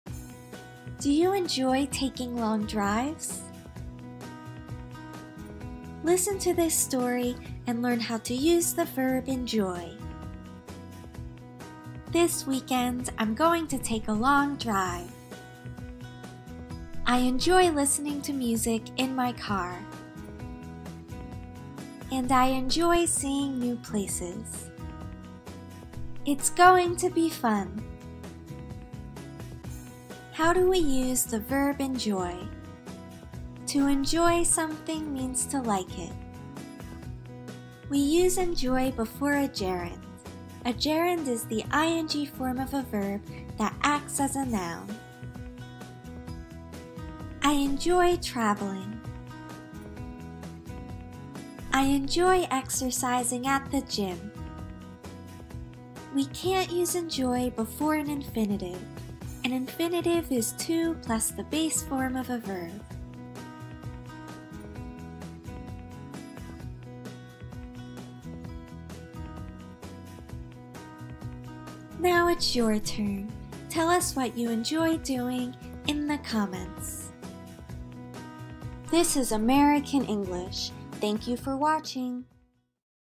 English Language Grammar: Gerunds and Infinitives after "Enjoy"
What do you enjoy doing in your free time? If you enjoy learning about English grammar, check out this video. It explains how to use the verb “enjoy.” We hope you enjoy watching!